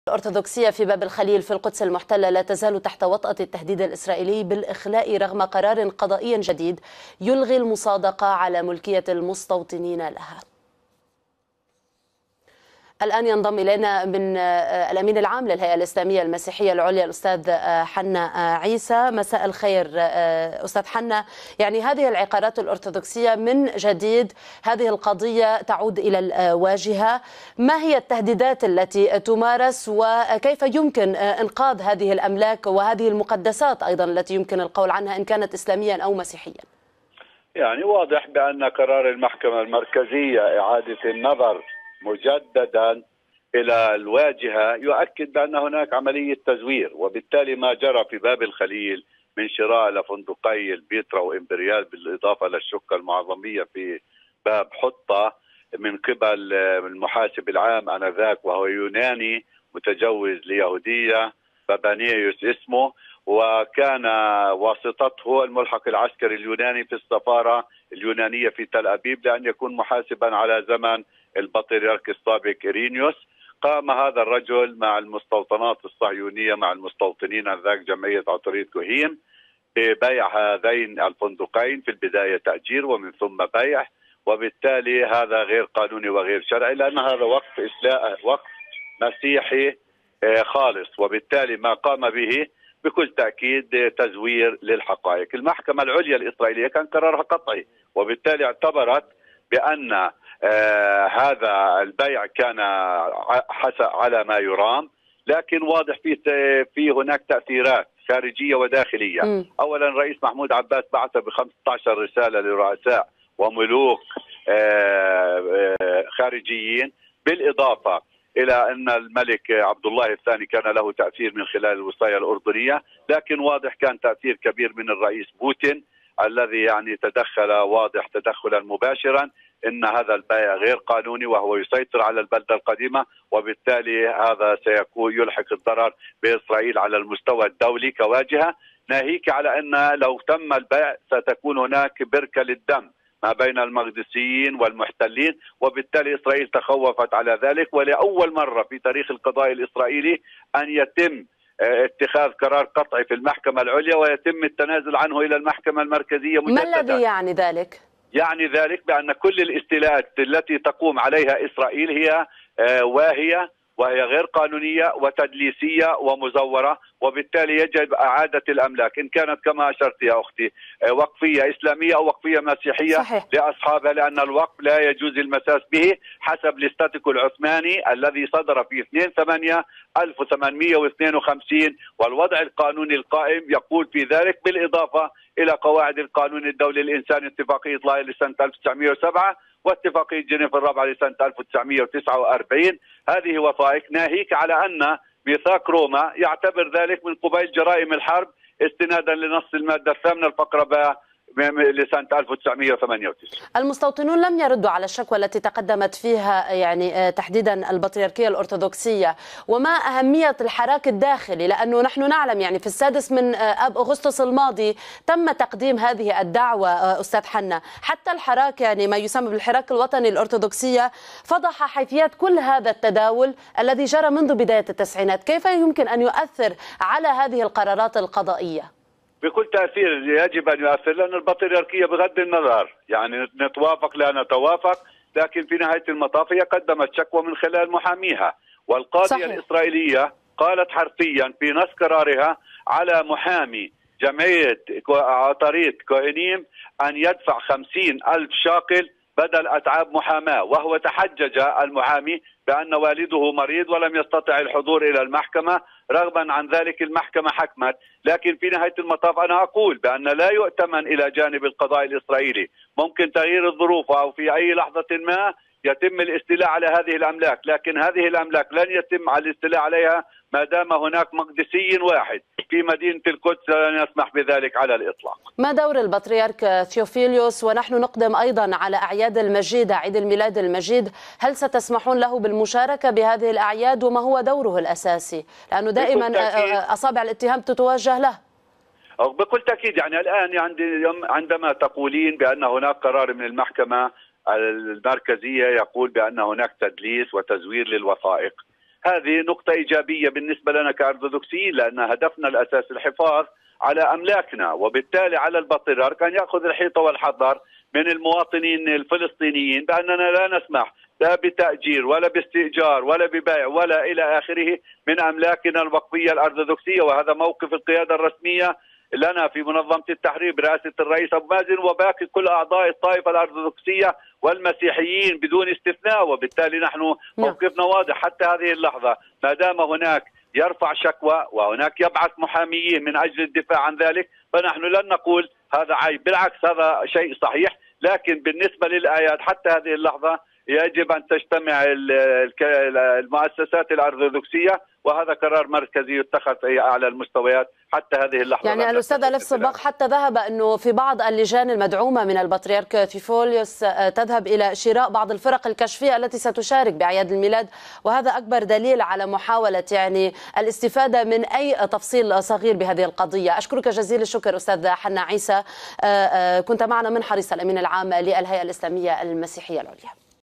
مقابلة الميادين مع الأمين العام للهيئة الإسلامية المسيحية لنصرة القدس والمقدسات حنا عيسى
بعد اعتراض شريحة كبيرة من الفلسطينيين على بيع البطريركية الأرثوذكسية في فلسطين أملاكا وقفية، تقدمت البطريركية بدعوى استرجاع الأملاك. وبضغط فلسطيني وأردني وعالمي، أذعنت المحكمة الاسرائيلية، وأصدرت قرارا بالتراجع عن المبيعات التي حدثت.